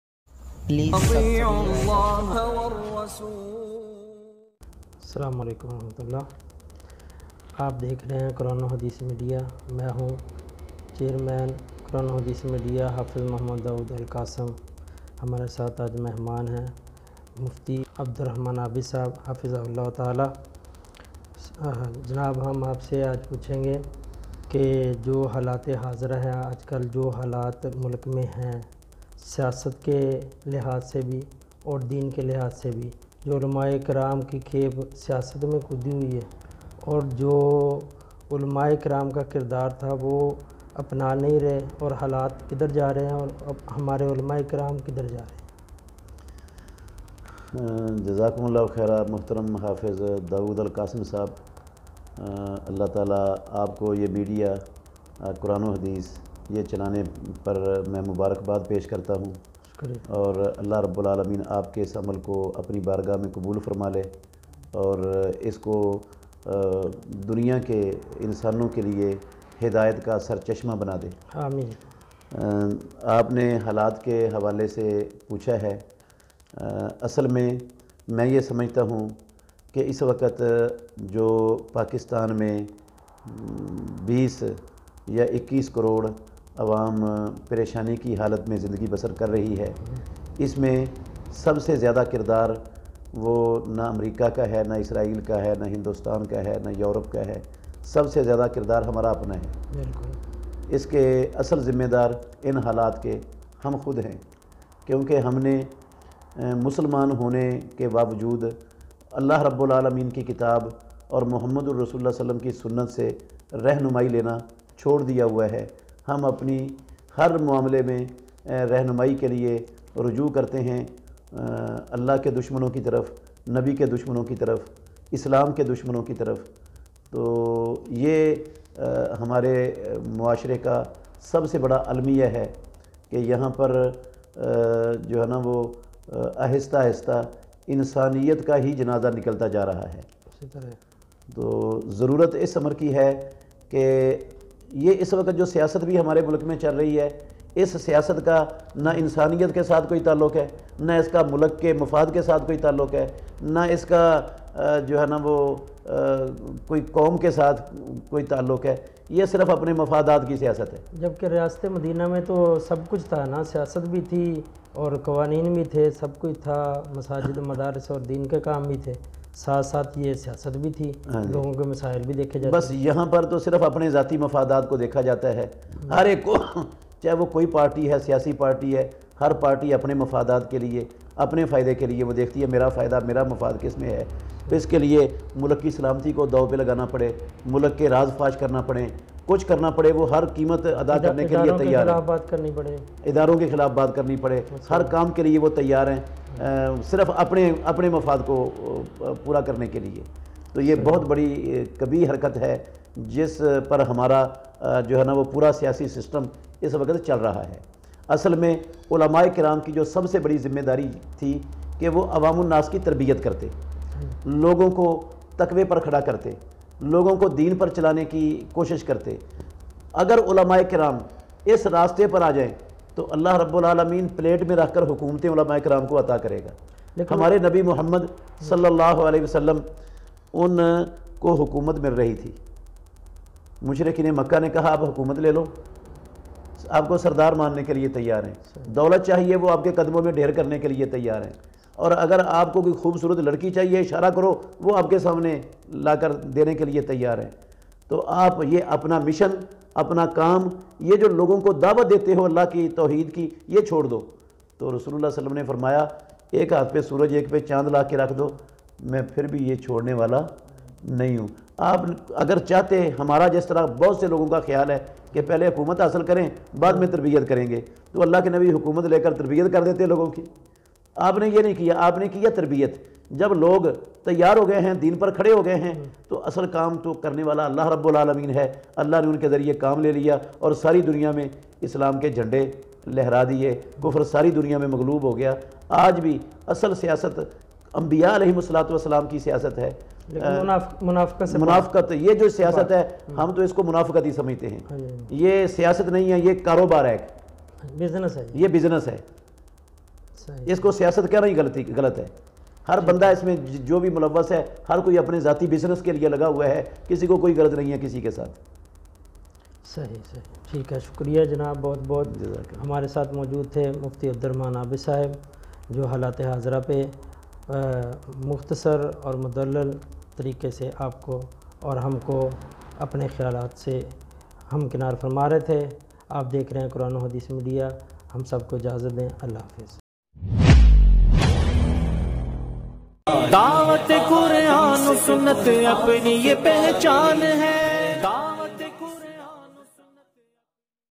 Mufti Abdul Rahman Abid | Great conversation on the situation | QHM Official Video
مفتی عبدالرحمن عابد صاحب کی حالات حاضرہ پر بہت زبردست گفتگو

Quran Wa Hadees Media Pakistan
قرآن و حدیث میڈیا ٹیم پاکستان